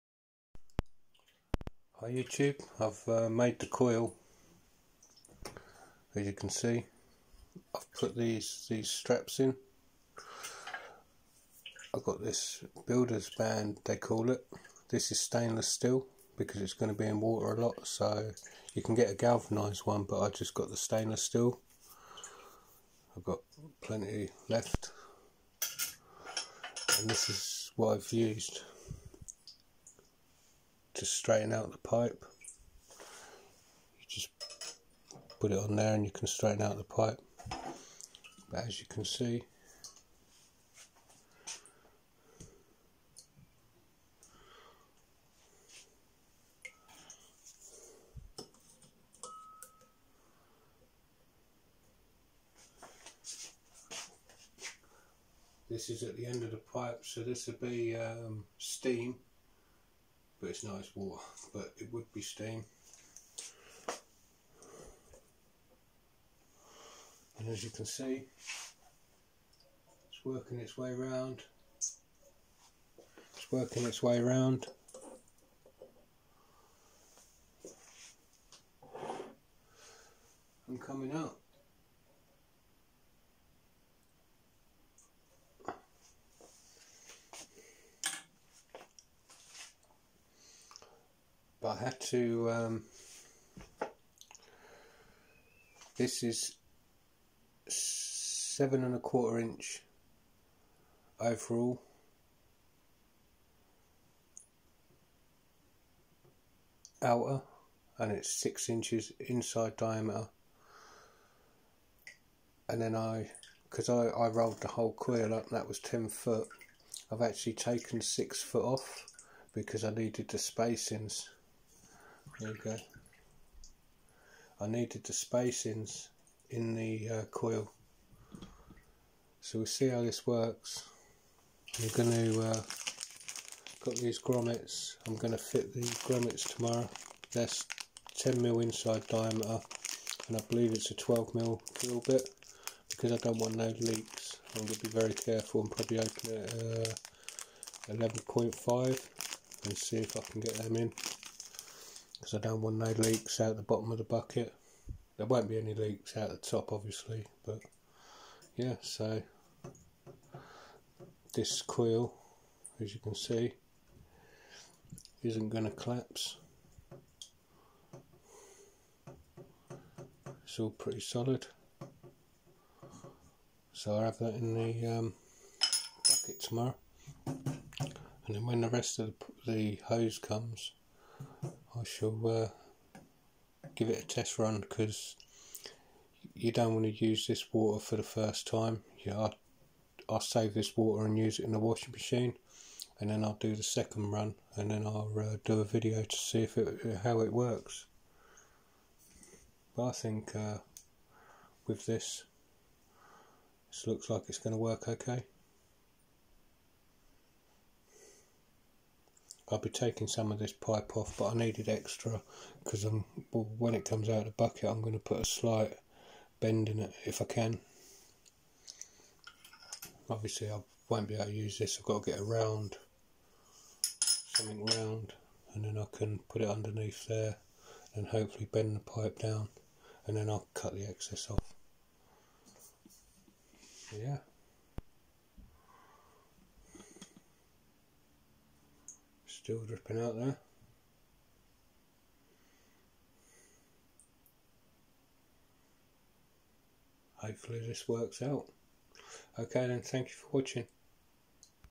Home Made Water Destiller. Sorted The Coil Out Today. Part 2.
I have been working on my water distiller coil today. I used Sabrefix builders band stainless steel to fix the coil so it wont move when fitted to the bucket. I will fit the coil in the 25L bucket tomorrow.